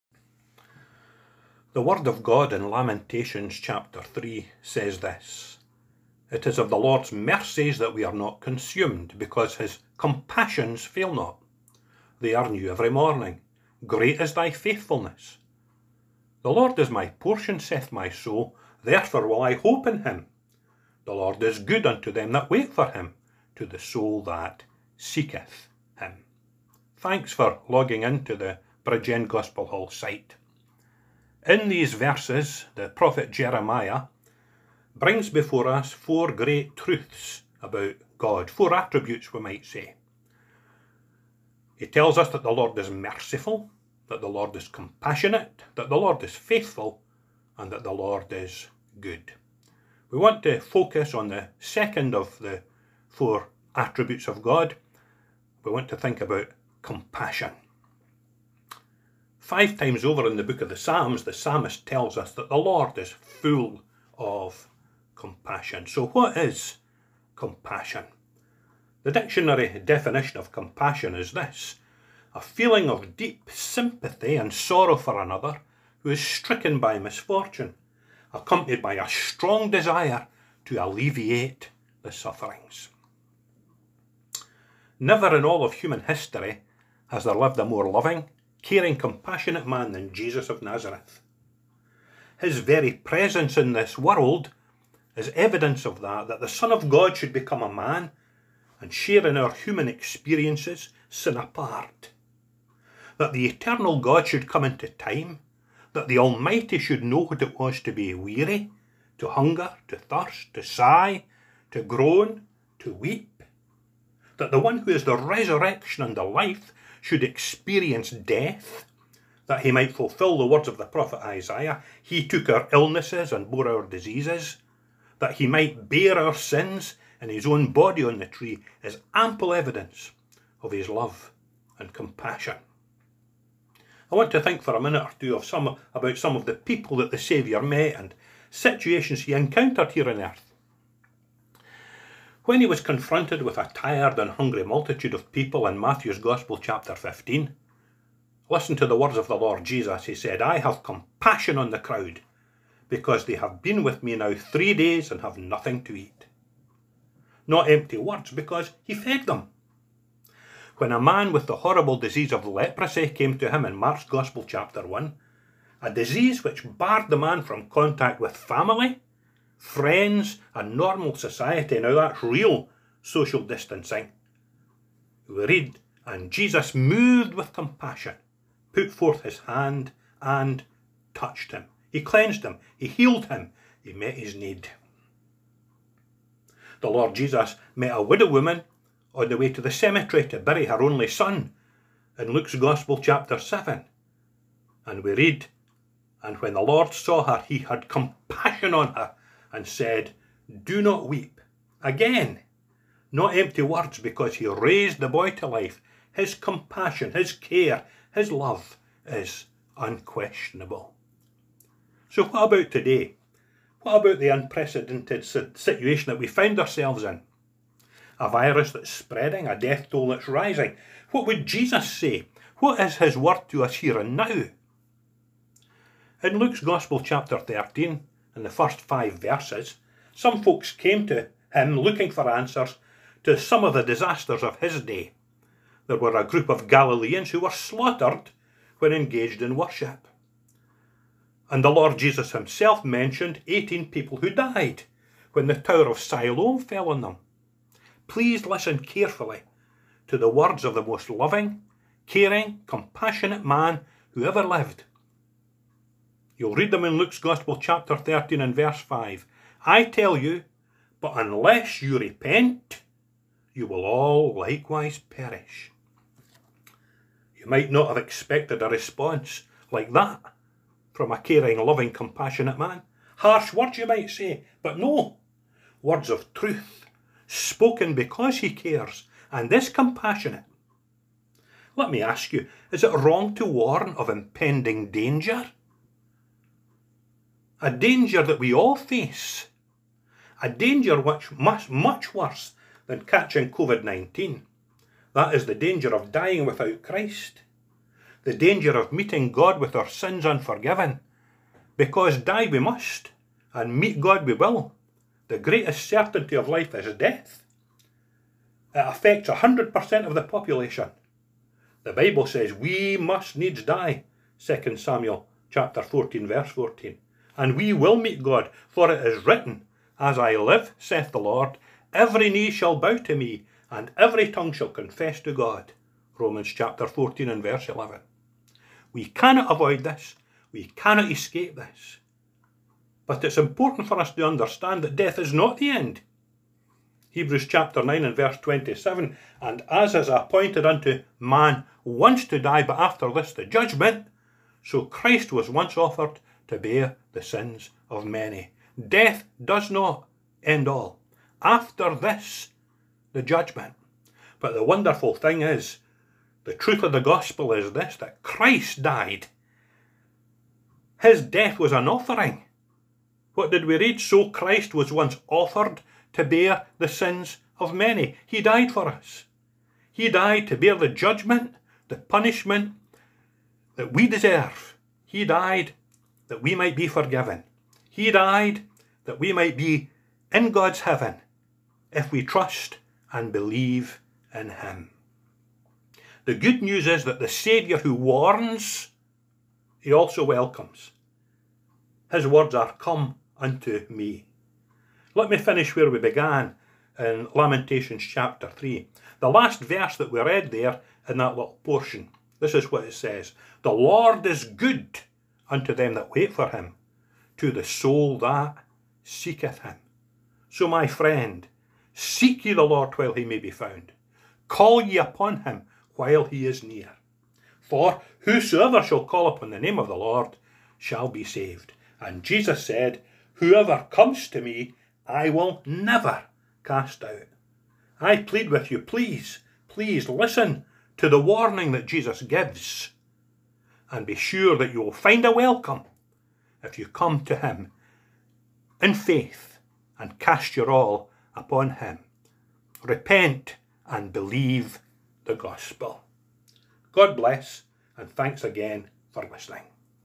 5th April 2020 - The Compassion of the Lord
Todays gospel presentation from Scott Meikle focuses on the compassion that God has for us...